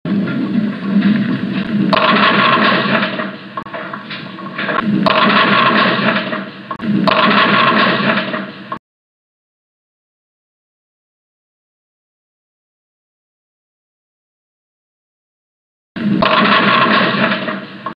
Runaway cart mows down people at game like bowling 2
Runaway cart mows down people at game like bowling a strike